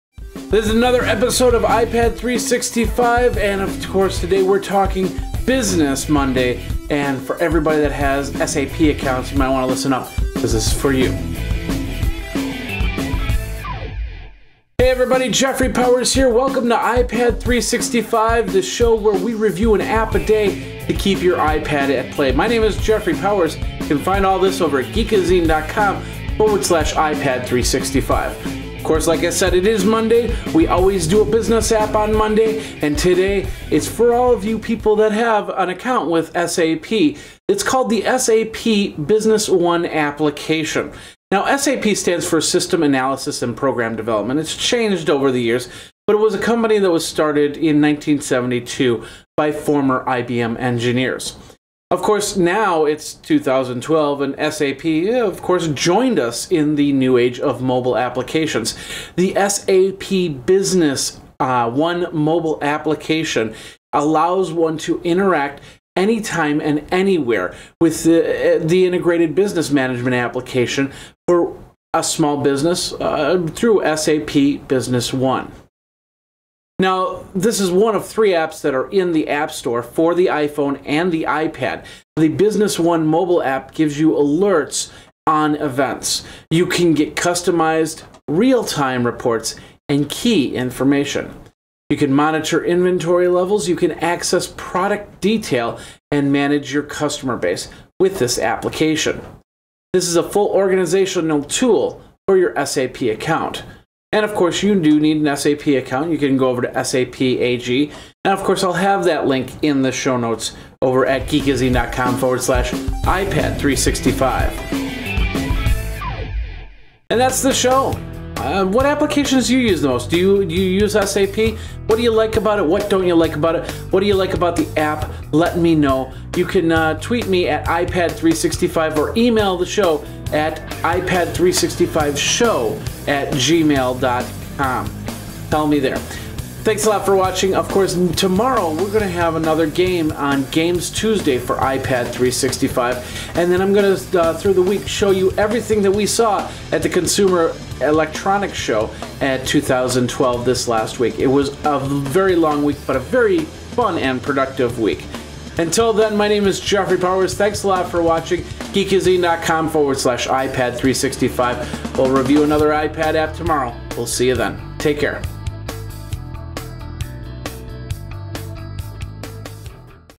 SAP Business One Mobile App
For those of you who use SAP. This app organizes data and gives you information at your fingertips.